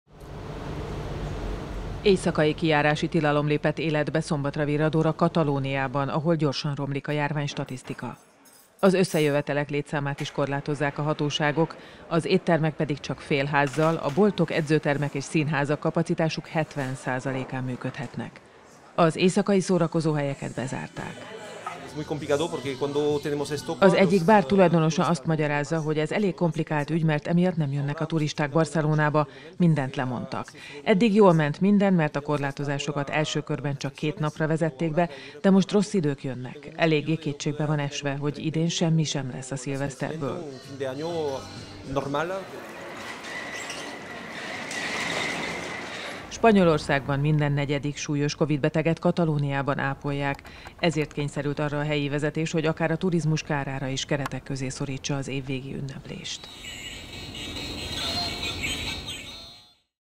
Éjszakai kijárási tilalom két héten át Katalóniában
Az éttermek, edzőtermek, színházak is csak csökkentett kapacitással tarthatnak nyitva, az éjszakai szórakozó-, illetve vendéglátóhelyeket pedig bezárták.